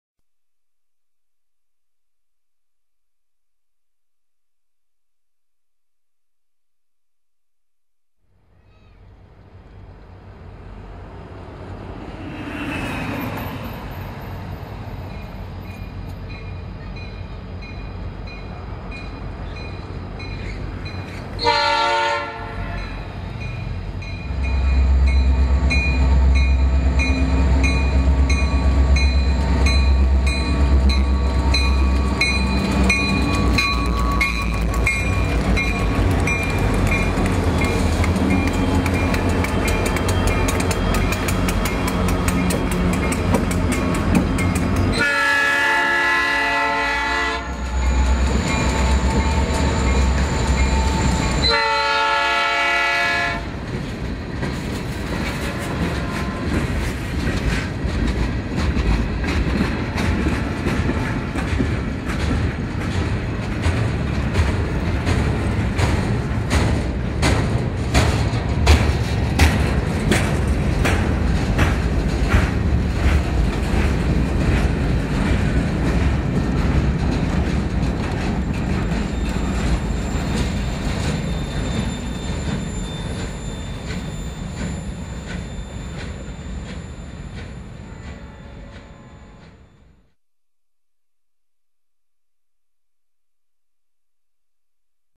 INRD 1701 in Bedford
Coming back home from Paducah with a sparkly new paint job.